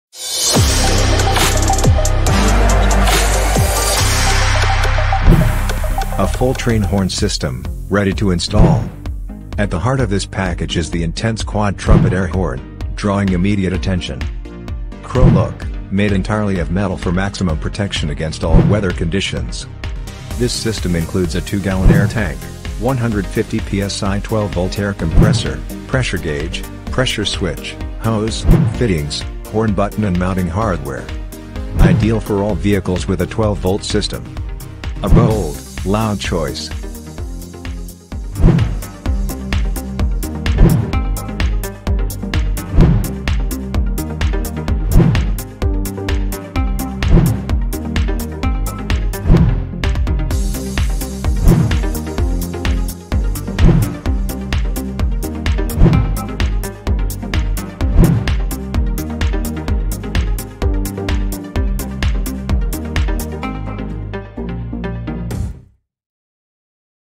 📢 VIXEN HORNS VXO8560/4318C Full Train Air Horn System Kit 🔥
⭐ Brand: VIXEN HORNS™
⭐ Model: VXO8560/4318C
⭐ Product: Full Train Air Horn System Kit

✅ Quad Trumpet Train Air Horn

✅ Tank Capacity: 2 Gallon (8 liter)

✅ 12 volt 150 PSI Compressor

✅ Thermal Overload Protection

✅ Mounting Hardware Included

A full train horn system, ready to install.

At the heart of this package is the intense quad trumpet air horn, drawing immediate attention.

Chrome look, made entirely of metal for maximum protection against all weather conditions.

This system includes a 2 Gallon air tank, 150 PSI 12 volt air compressor, pressure gauge, pressure switch, hose, fittings, horn button and mounting hardware.

Ideal for all vehicles with a 12 Volt system.

A bold, loud choice.